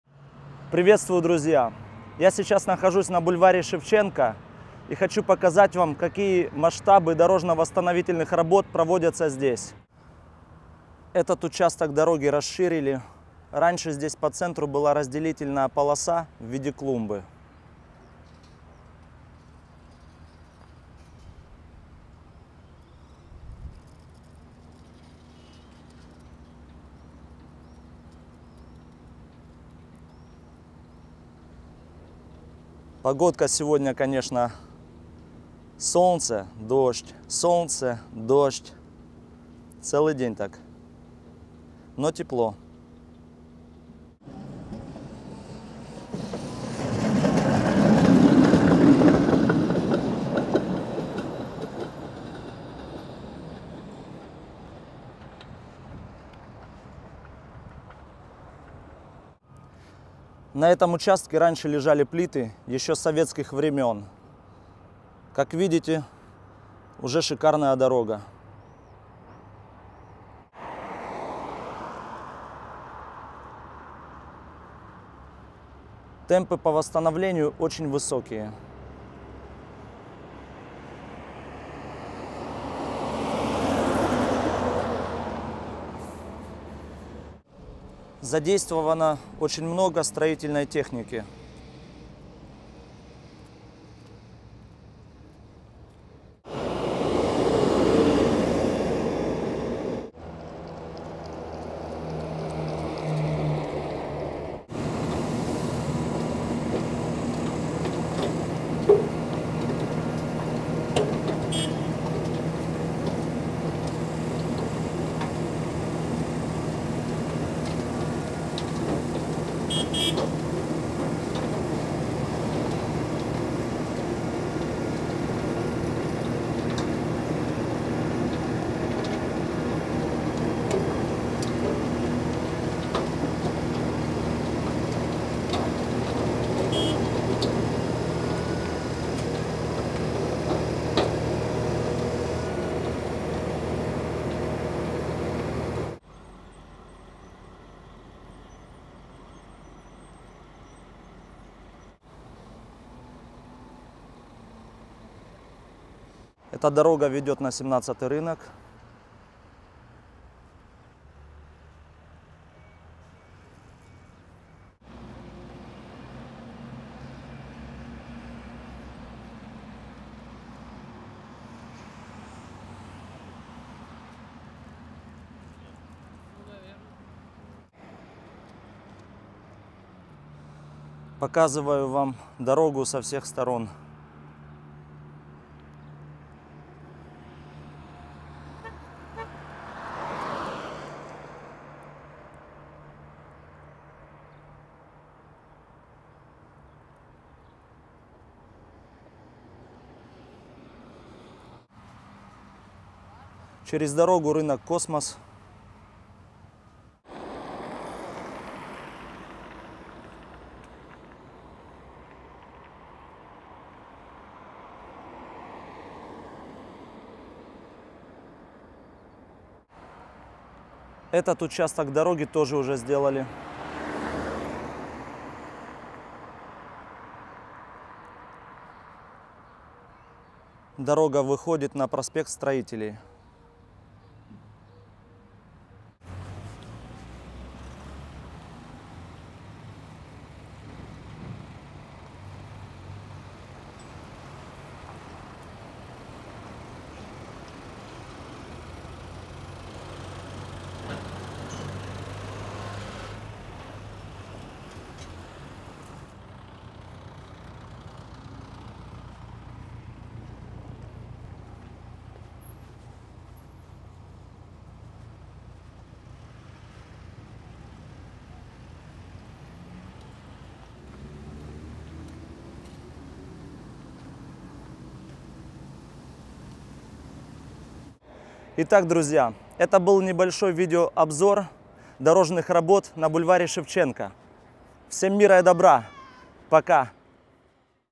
Шикарные дороги в Мариуполе. Бульвар Шевченко.
Друзья, дороги в Мариуполе выходят на другой уровень! Рекомендую к просмотру! Всем мира и добра! Благодарю Вас за поддержку!